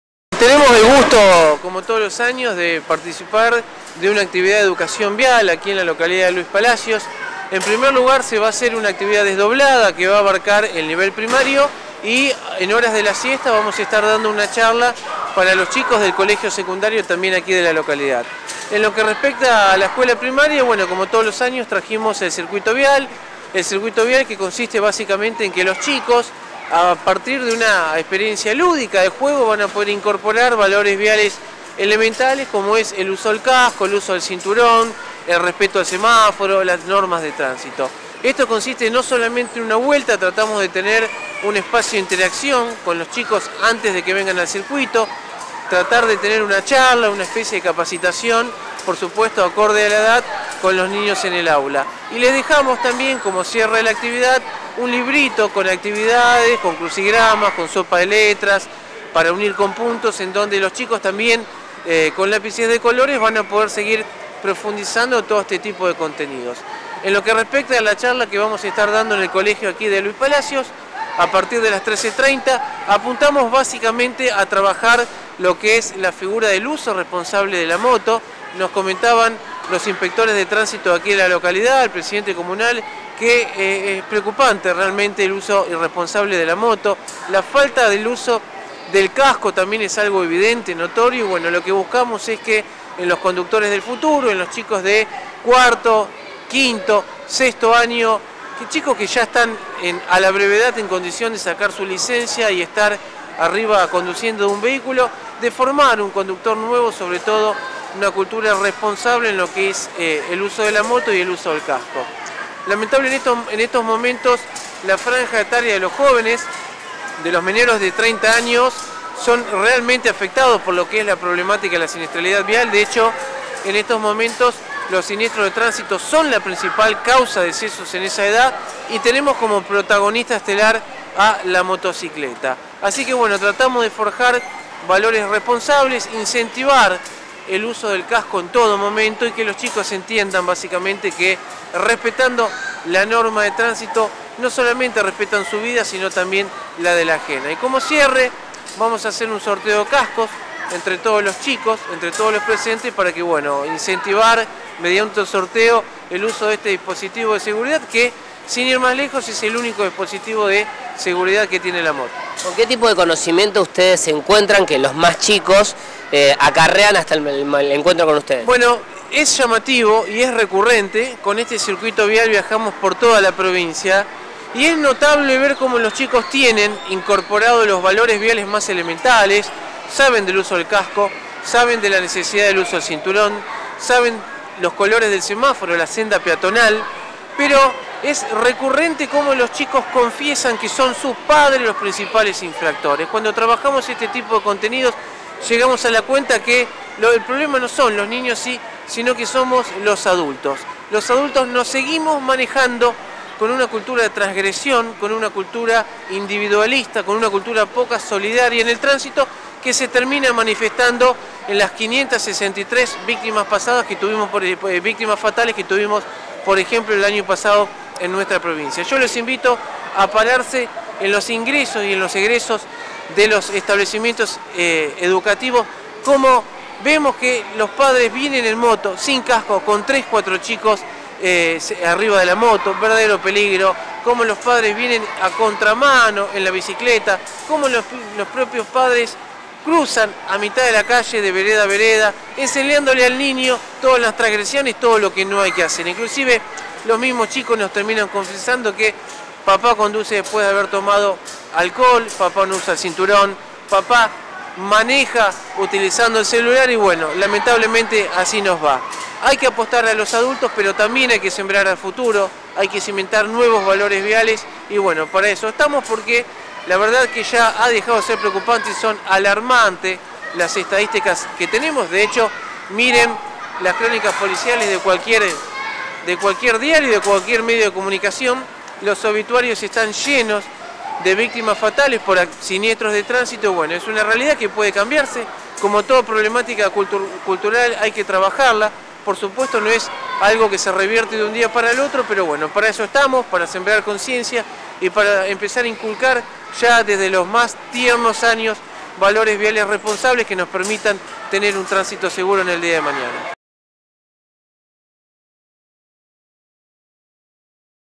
Jornada desarrollada por la APSV para niños palacenses
La Agencia Provincial de Seguridad Vial estuvo presente en Luis Palacios desarrollando una nueva jornada de enseñanza de normas viales a los más pequeños.